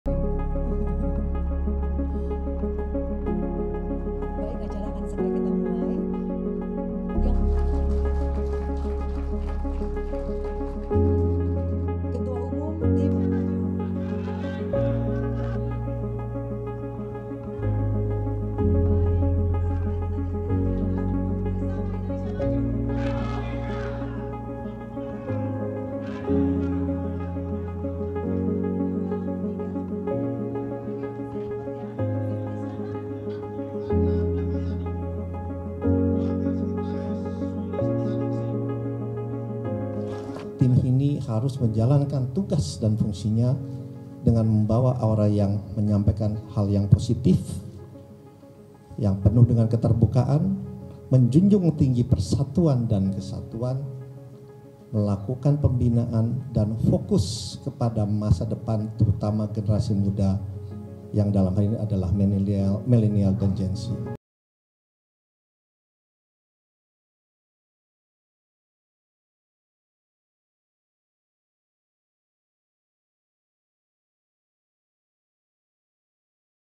Tim Kampanye Nasional Prabowo Gibran Diresmikan, Ini Para Anggotanya
TEMPO.CO - Tim Kampanye Nasional Koalisi Indonesia Maju yang mengusung Prabowo Subianto-Gibran Rakabuming Raka resmi dibentuk. Ketua Tim Kampanye Nasional, Rosan P Roeslani, dan Sekretaris TKN, Nusron Wahid, mengumumkan susunan struktur lengkap tim kampanye, Senin 6 November 2023. (Ibnu Zaki/Fauzan/Rizky Bagus Dhermawan/Nanien Yuniar)